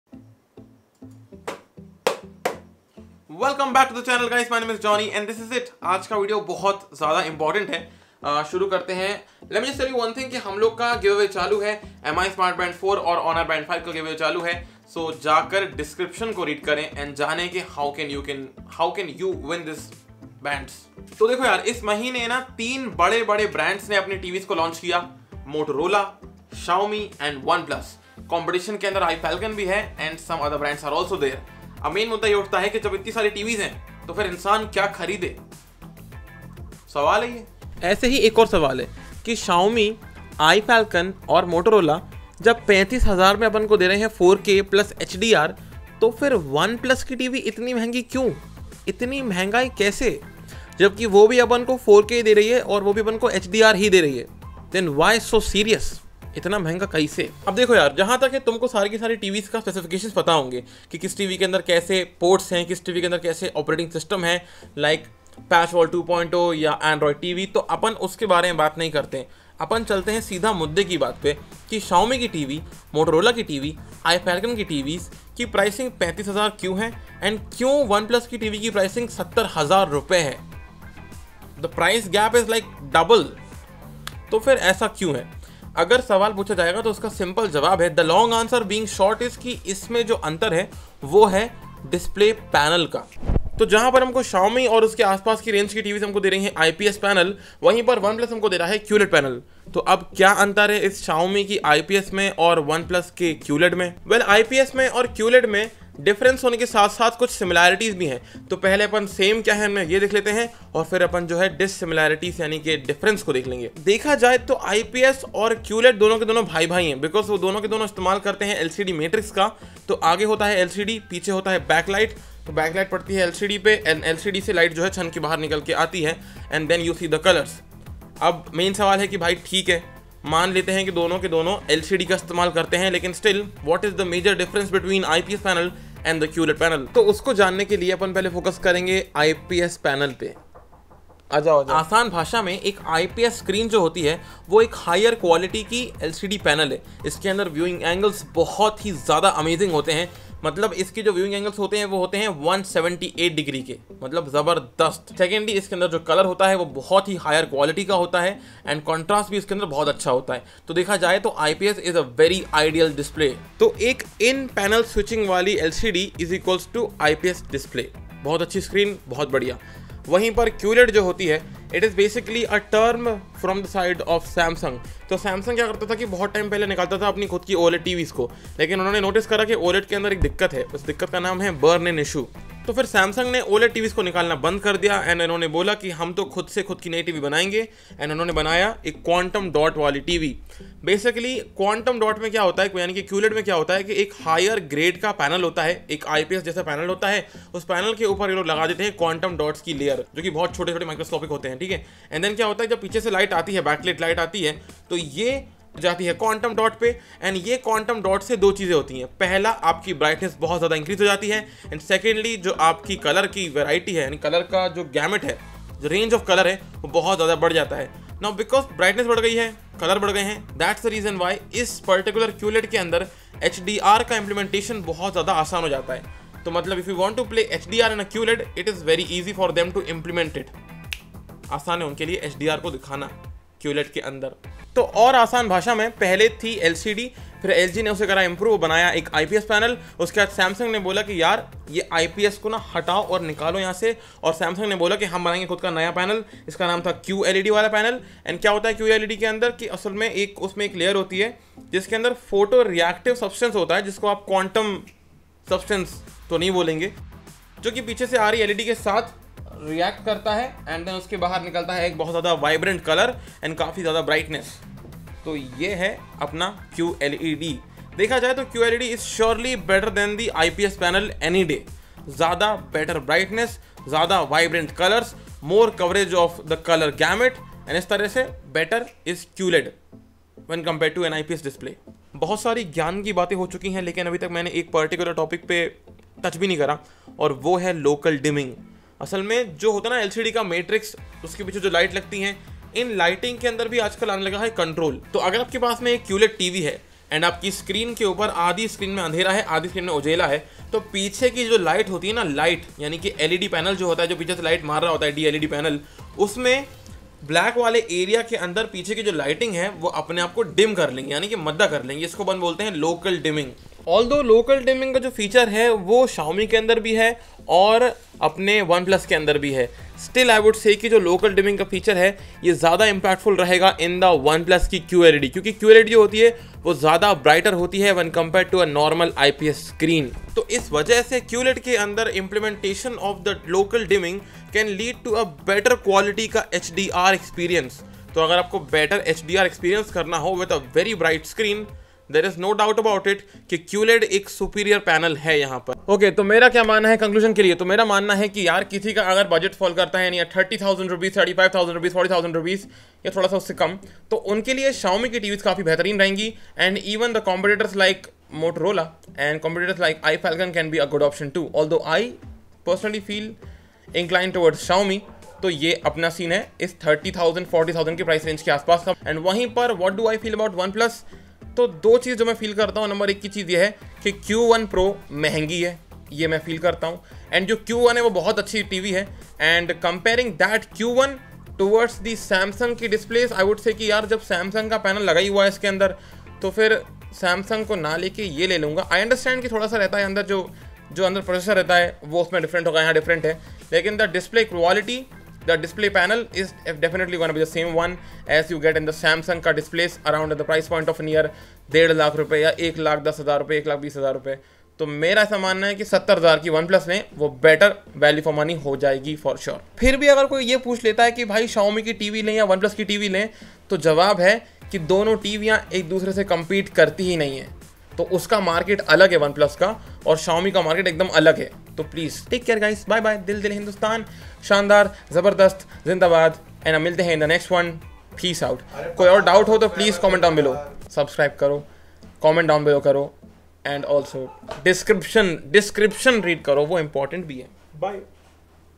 One Plus VS MI TV || IPS vs QLED || things you must know.
Comparing IPS vs QLED | ONE PLUS TV vs MI TV 4X . What should you buy ?

HOW TO TAKE PART IN THE GIVEAWAY (all rules apply to giveaway video)

4. FOLLOW ON INSTAGRAM